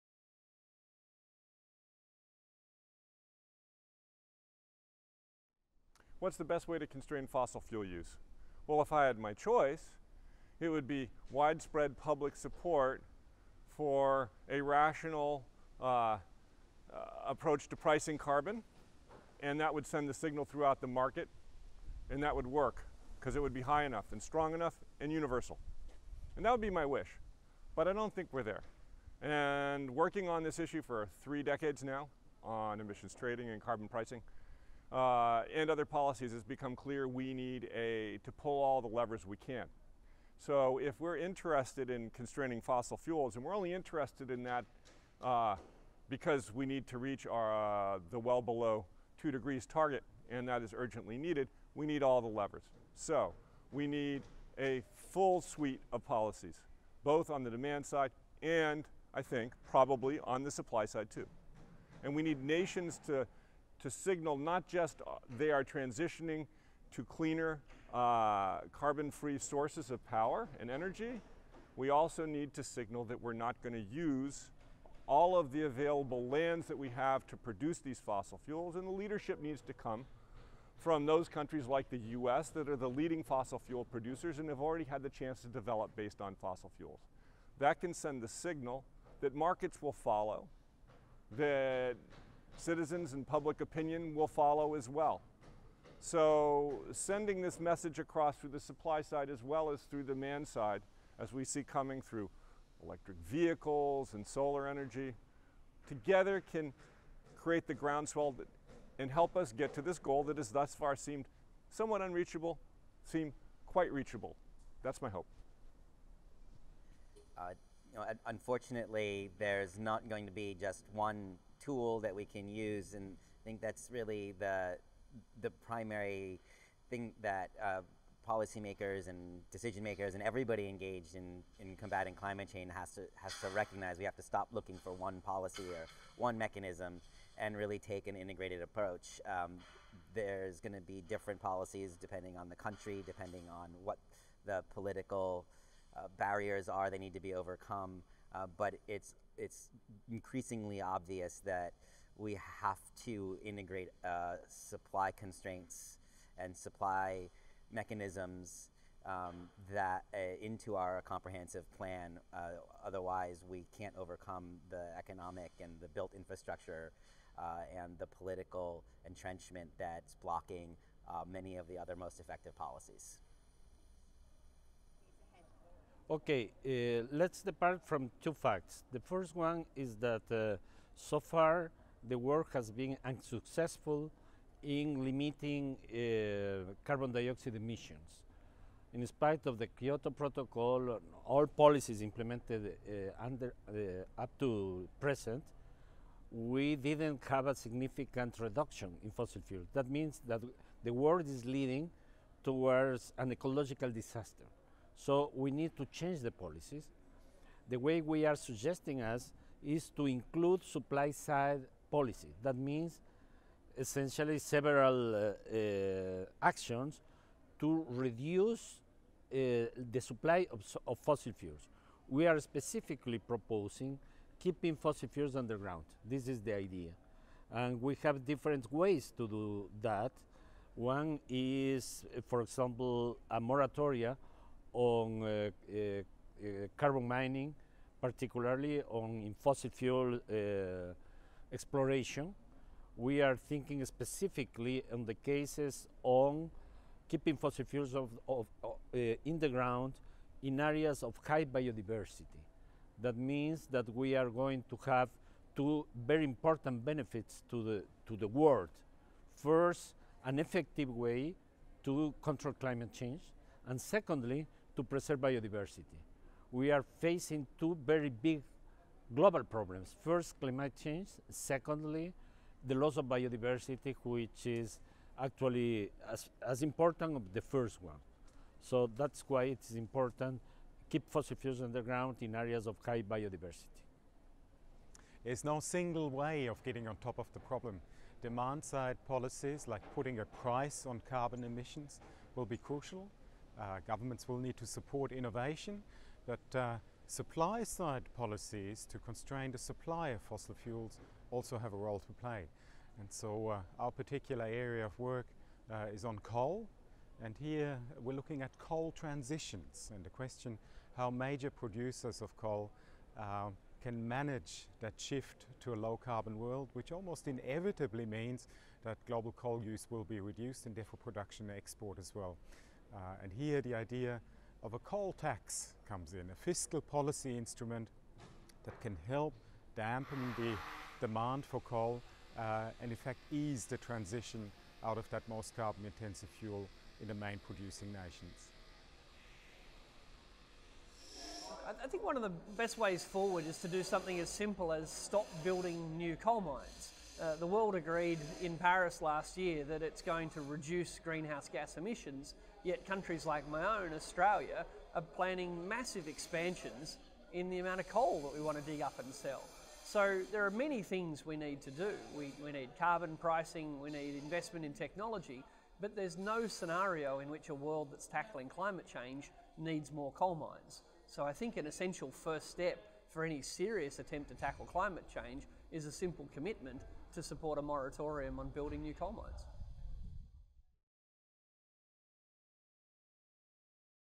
What’s the best way to constrain fossil fuels in the decades ahead?
Carbon Brief asked a number of experts what they thought was the best way to limit fossil fuel use, a conference on fossil fuel supply and climate policy in Queen's College, Oxford.

Speakers are, in order of appearance:

- Michael Lazarus, Senior Scientist at the Stockholm Environment Institute

- Franz Matzner, Director of the National Resources Defense Council’s Beyond Oil Initiative

- Prof Carlos Larrea, professor of Social Studies at the Universidad Andina Simón Bolívar

- Frank Jotzo, associate professor at and Deputy Director of Crawford School of Public Policy, Australian National University

- Richard Denniss, chief economist at The Australia Institute